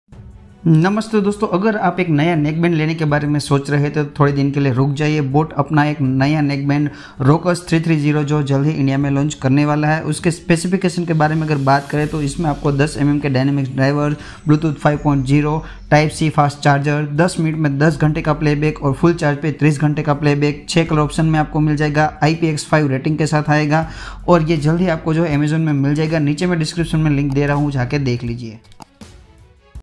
Wait For 🔥 New Boat Rockerz 330 Nack Band.
New neckband bluetooth headset by boat. Company Launch his new Rockerz 330 Neckband. Coming soon on Amazon web site. Rockerz 330 bluetooth nackband comes with 6 colour, 30 hours music play time, usb type c charger, 10 minutes charge play music 10 hours, one tuch assistant and many more. Rockerz 330 in full charge play music 30 hours.

Boat is leding company for neckband headphones or neckband earphones.